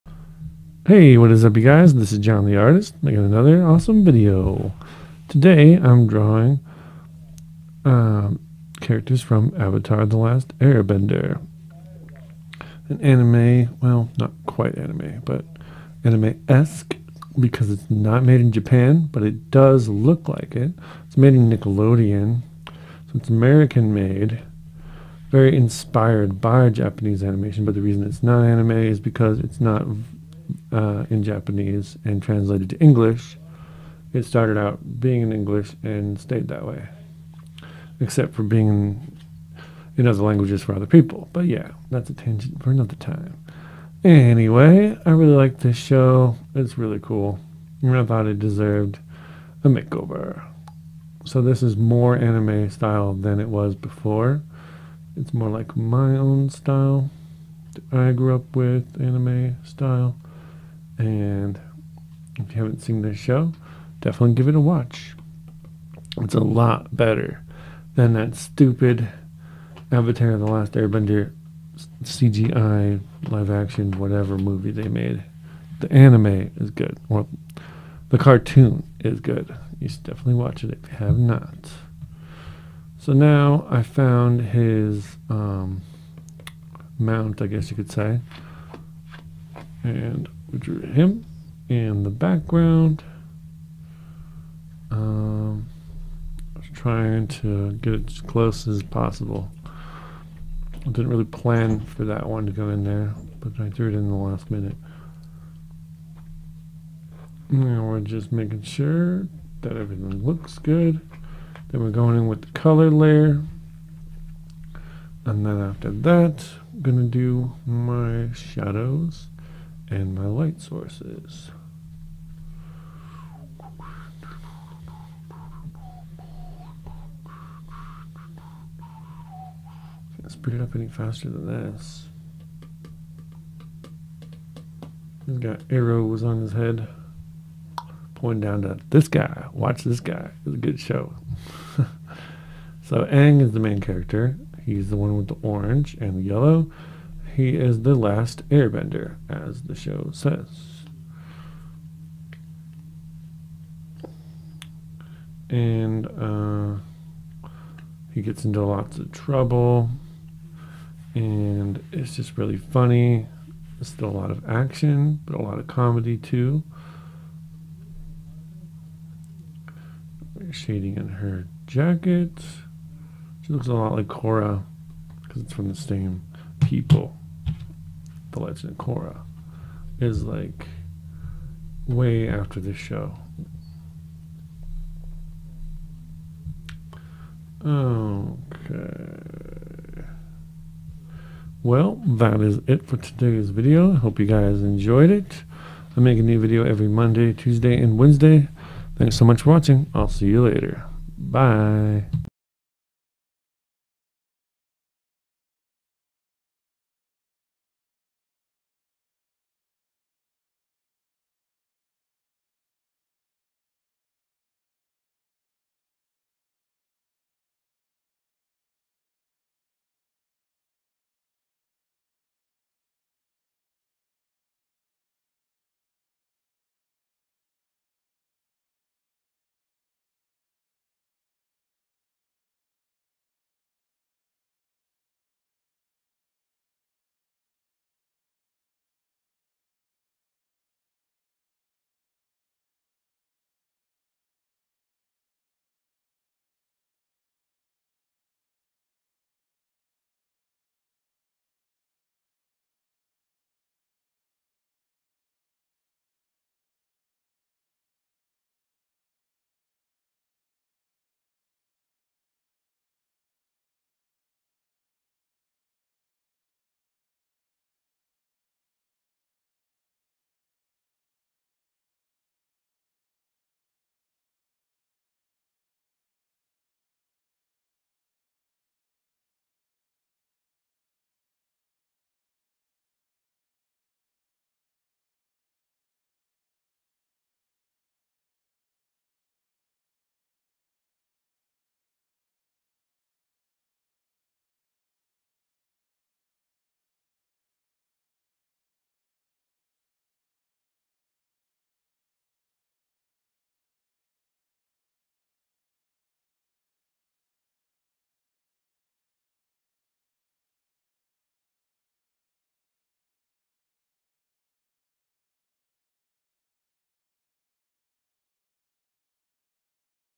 Avatar The Last Airbender
Hey Everybody it's Jon The Artist bringing you another awesome video, today I drew Avatar The Last Airbender